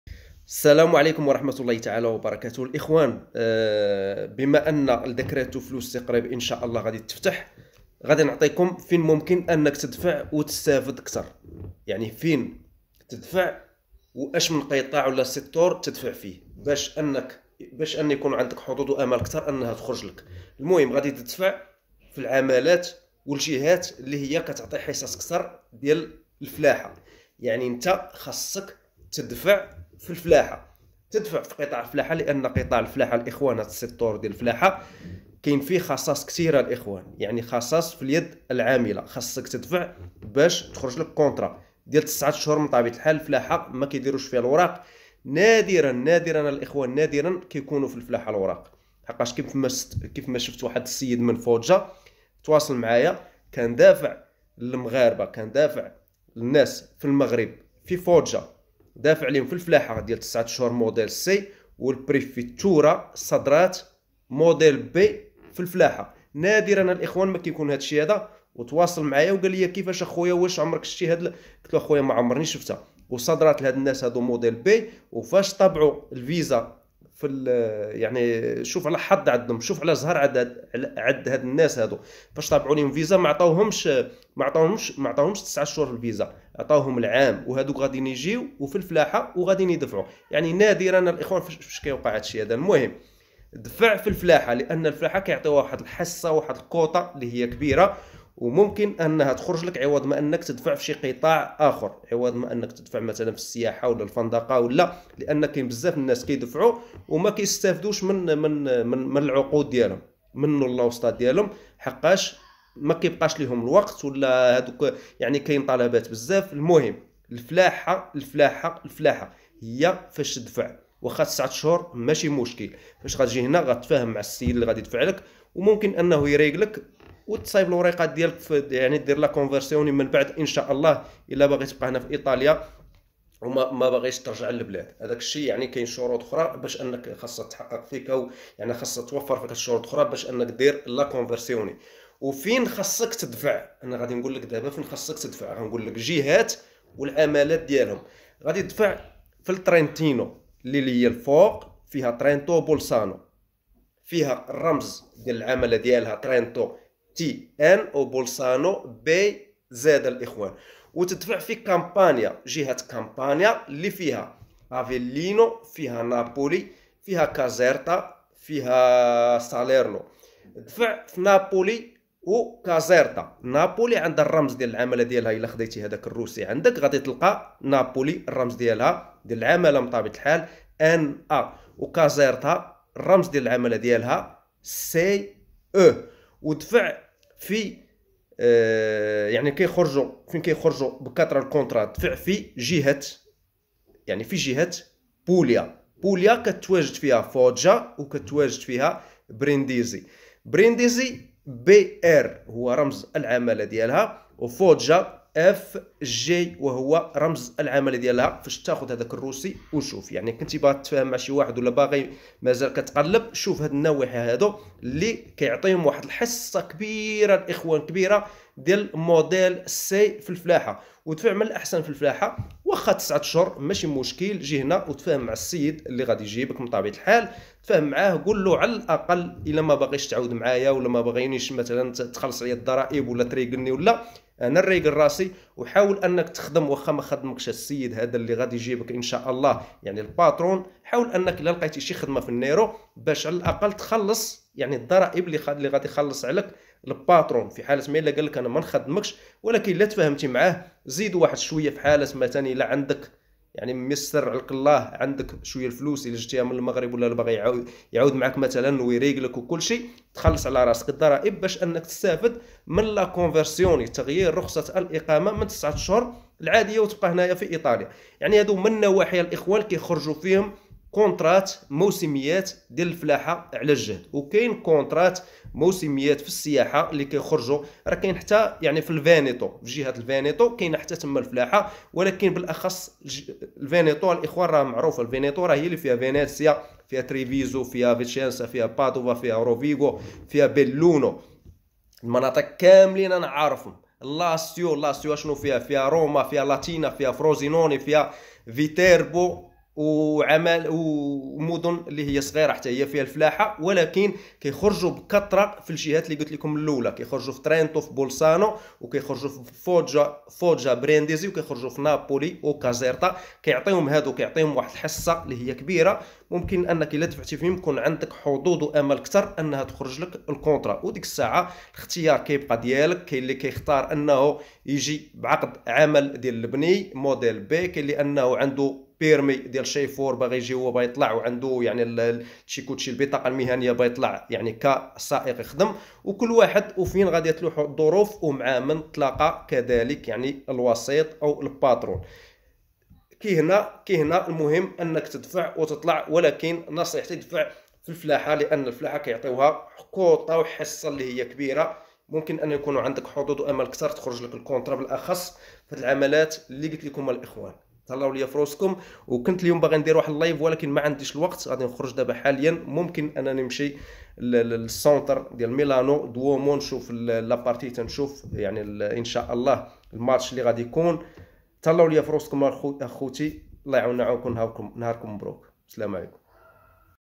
ديكريطو فلوسي: ها فين تدفع وها أشنو تدفع بحظوظ وامال كثر تخرج ليك لكونطرا
تواريخ متداخلة لإرسال الطلبات مع الموعد النهائي في 17 مارس 2022 والوصول إلى الإجراء باستخدام بيانات اعتماد Spid.
يتم تقسيم الأسهم المتبقية بين إدخالات المواطنين من خارج الاتحاد الأوروبي الذين أكملوا برامج التدريب والتعليم في بلدانهم الأصلية ، وإدخالات العمال من أصل إيطالي المقيمين في فنزويلا وإدخالات المواطنين من خارج الاتحاد الأوروبي للعمل لحسابهم الخاص وكذلك بين التحويلات من تصاريح الإقامة الممنوحة بالفعل لأسباب أخرى في تصاريح الإقامة للعمل الثانوي والعمل الحر. سيكون من الممكن إرسال الطلبات من 9.00 يوم 27 يناير 2022.
سيتم تقسيم الحصص ، من قبل وزارة العمل والسياسات الاجتماعية ، بين المناطق والأقاليم المتمتعة بالحكم الذاتي. سيكون من الممكن إرسال الطلبات من الساعة 9.00 في 1 فبراير 2022. سيكون تطبيق الملء المسبق لاستمارات الطلب متاحًا من الساعة 9.00 في 12 يناير 2023 ، من خلال الوصول إلى إجراءات الكمبيوتر المشار إليها على موقع الويب